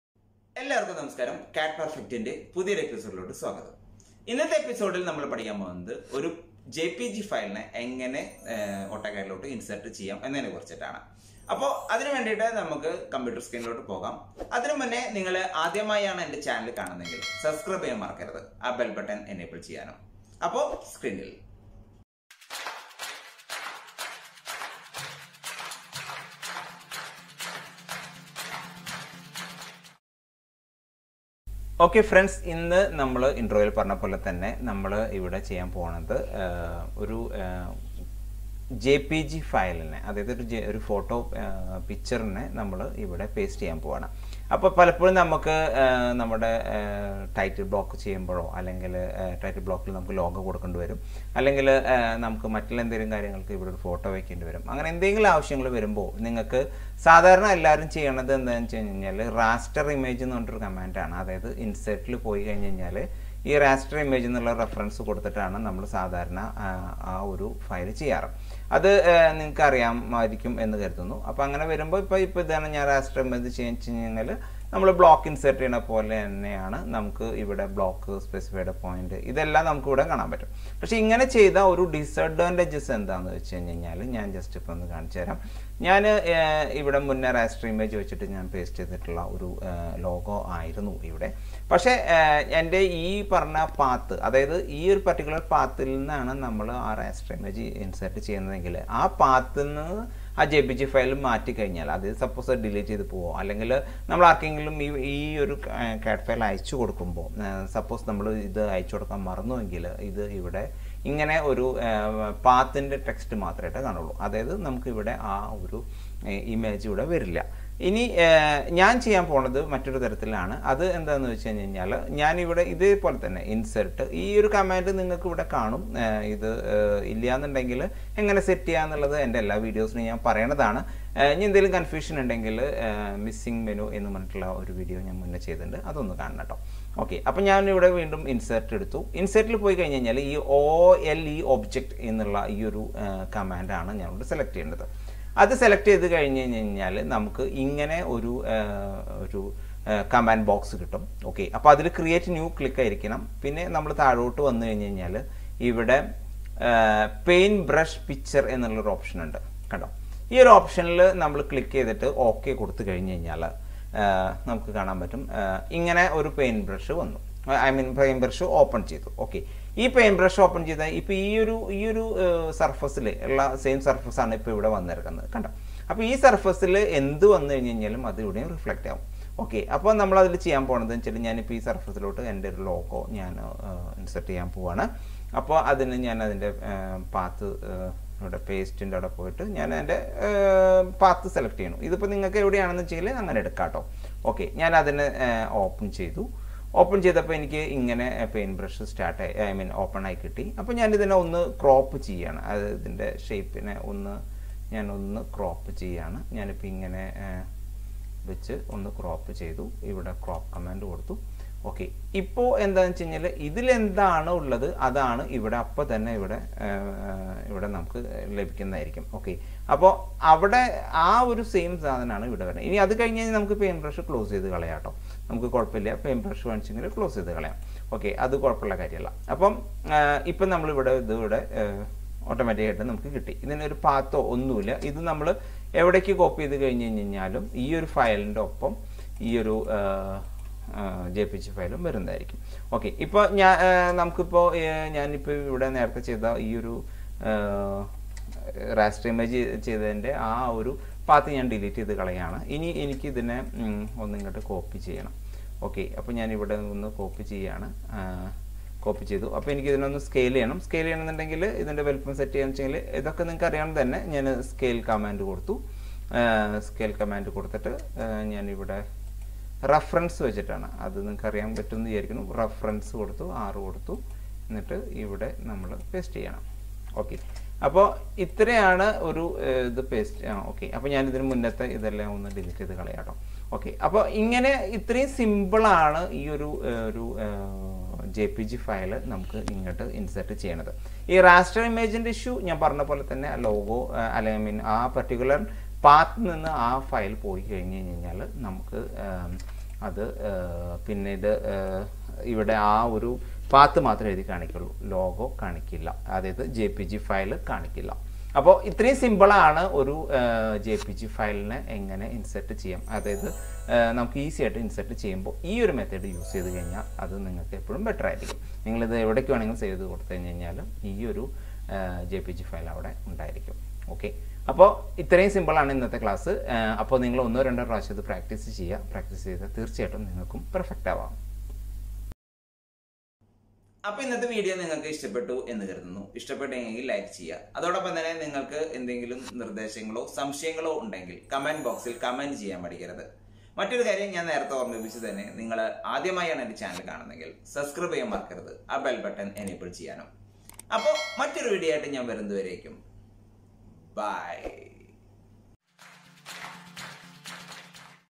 AutoCAD Malayalam | Raster Image | How to Insert a jpg to Auto CAD | Easy Malayalam Tutorial
Hi Friends, Here we can learn all about How to Insert a jpg File to Auto CAD. Please practice one or two times, you will be perfect